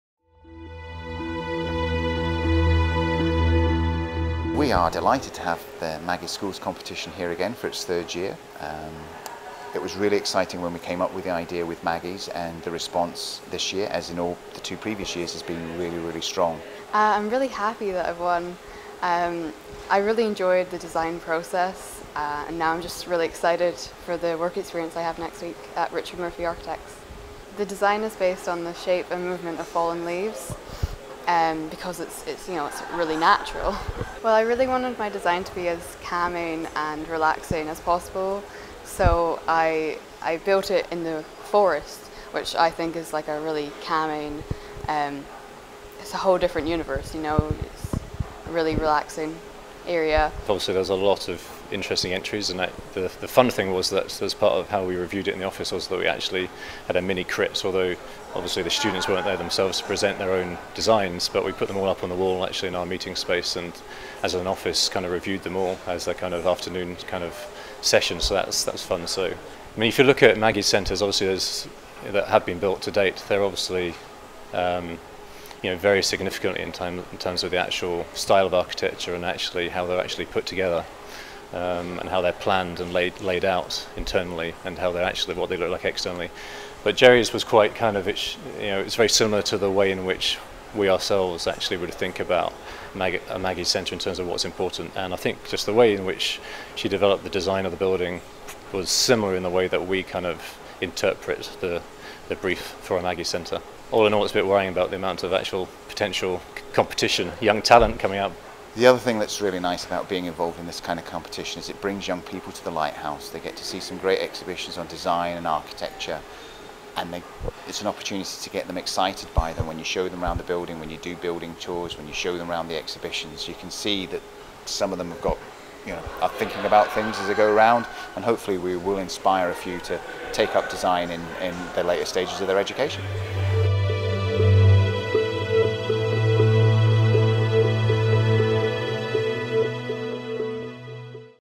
The Lighthouse: Maggie's Schools Architecture competition
Maggie's Schools Architecture Competition invited students to come up with their own plan for a Maggie's Centre.

Gerry Reid, a 5th year pupil at Culloden Academy, Inverness, won this year's national competition with her design inspired by the shape and structure of a leaf.

Judged by broadcaster Kirsty Wark and architect Richard Murphy, Gerry's prize is a week's work experience with Richard Murphy Architects, who created the first Maggie's Centre which opened in Edinburgh in 1996.